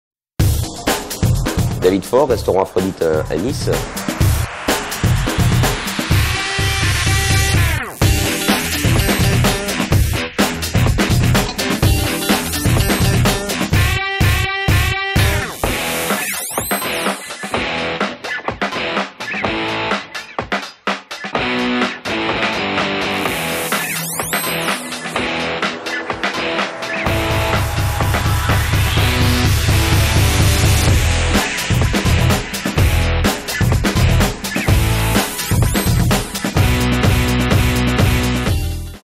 David Faure- Aphrodite- 1 Etoile au Michelin
Surprenante, inventive, atypique, gourmande, raffinée, entre simplicité et caractère... La cuisine de Davis Faure est à l'image de ce Chef qui a parcouru le monde. Toujours en recherche, toujours en quête de nouveautés, ce créatif a envie de renverser tous les codes et de bousculer les recettes traditionnelles afin de sublimer le produit et de le mettre en avant, grâce, entre autres, à la cuisine moléculaire.